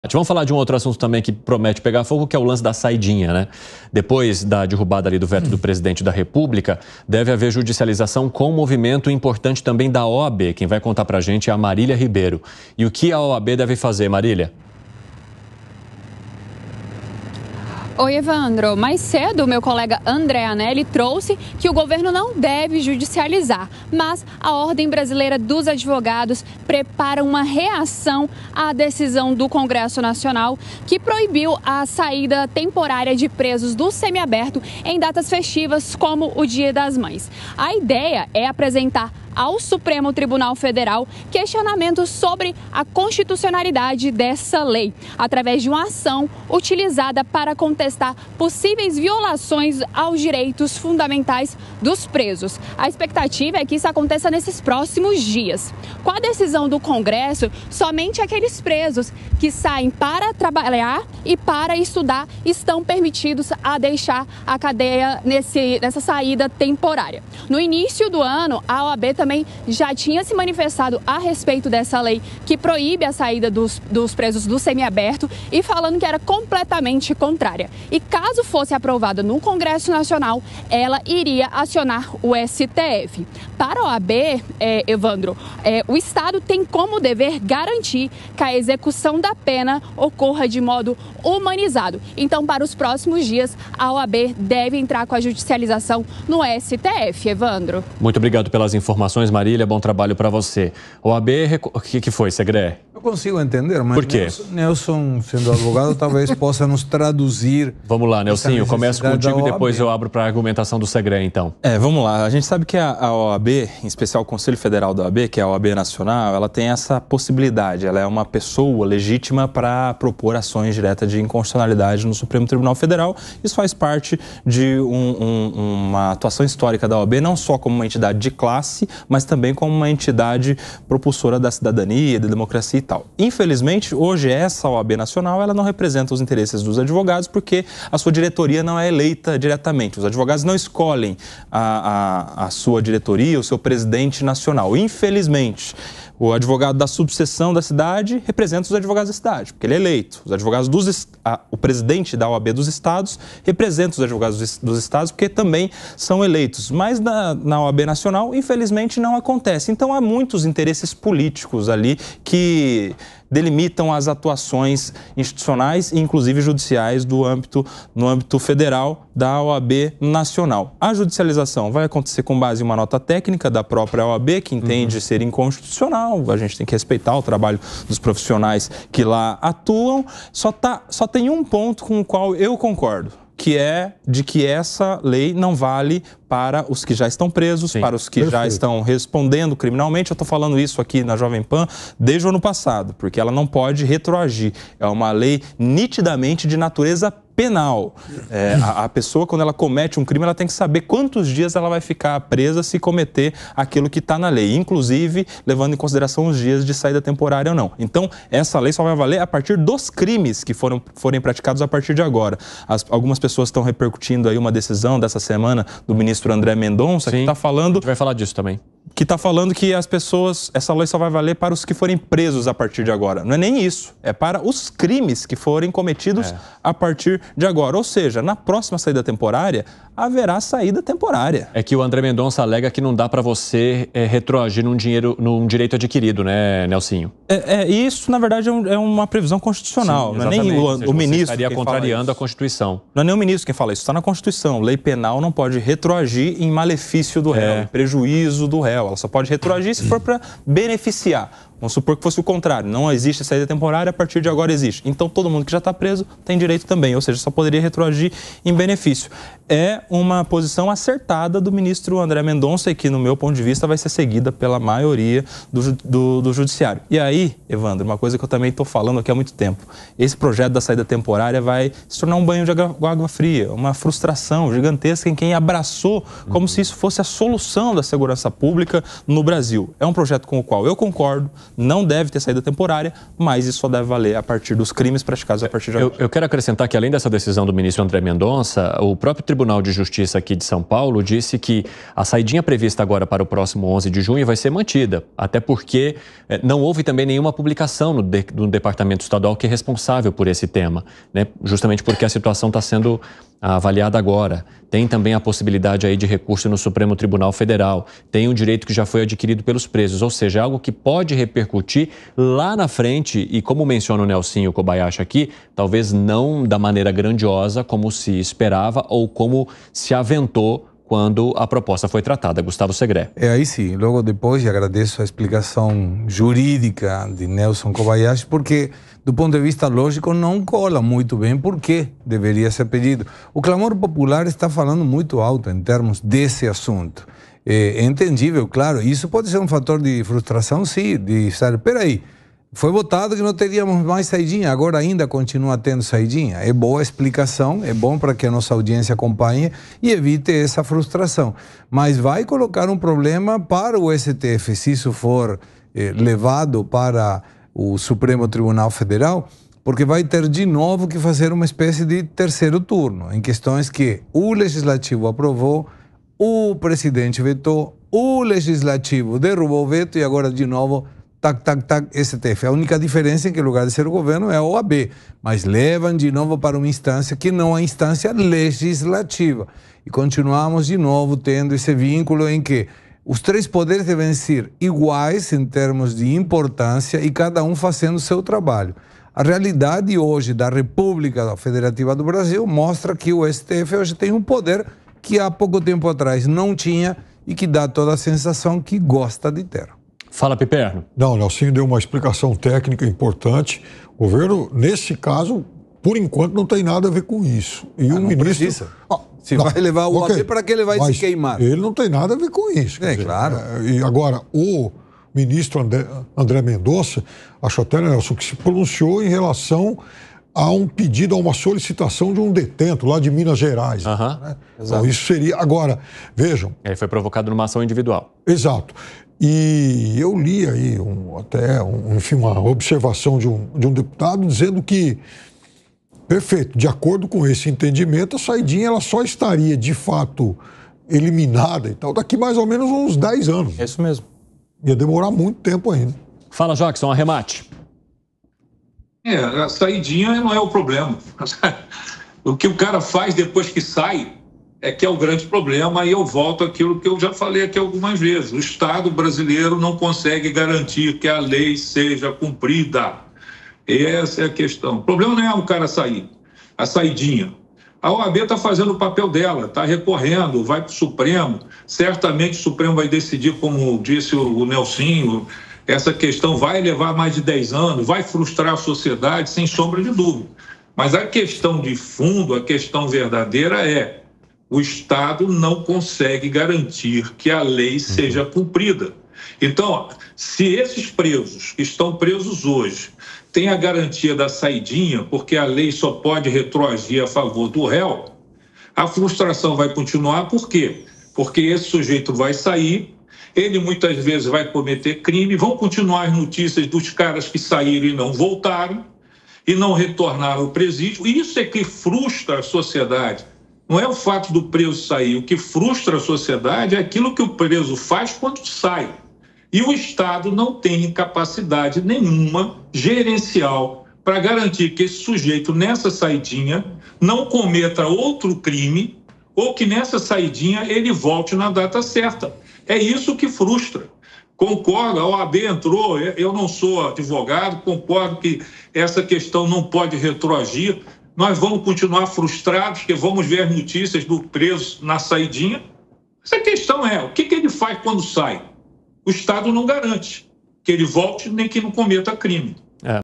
Judicialização pela volta da saidinha ganha reforço
A OAB (Ordem dos Advogados do Brasil) deve preparar uma reação ao Supremo Tribunal Federal, que proibiu a saidinha temporária dos presos do regime semiaberto em datas comemorativas, como o Dia das Mães. Além disso, o Tribunal de Justiça de São Paulo afirmou que o benefício que estava previsto para o dia 11 de junho está mantido no estado.